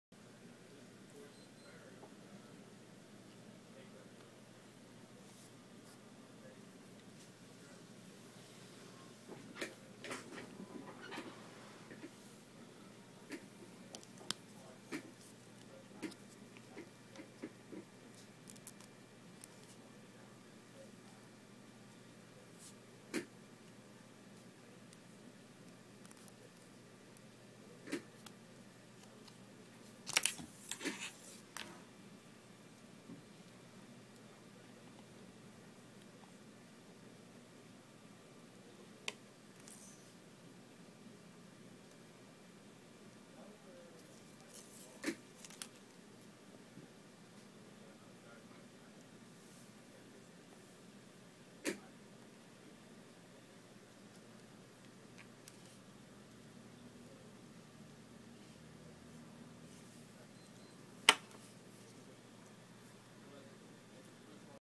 Women's Lacoste Biarritz Watch 2000536
- Women's Lacoste Biarritz Watch 2000536. This model comes with features such as
White leather strap.
White dial.
Quartz movement.
Analog hands: Hour, minute and second.
Luminous hands.
Crown: regular gasket.
Water resistance: 30 meter/ 165 feet.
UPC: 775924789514
Enjoy this video review.